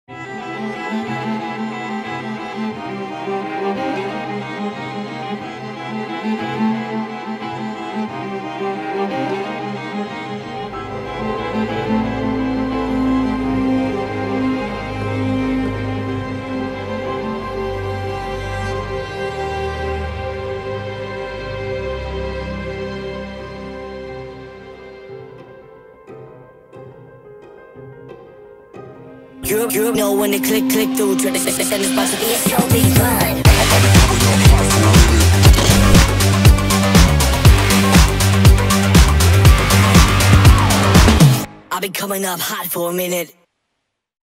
The show must go on! 📃✍️
Vorhang auf für weitere zwei Jahre!
Mit Kapitän Stefan Hierländer und Topscorer Jakob Jantscher verlängern zwei zukünftige Klublegenden ihre Verträge beim SK Puntigamer Sturm Graz bis 2023.

Vielen Dank an die Oper Graz für die Unterstützung bei der Umsetzung.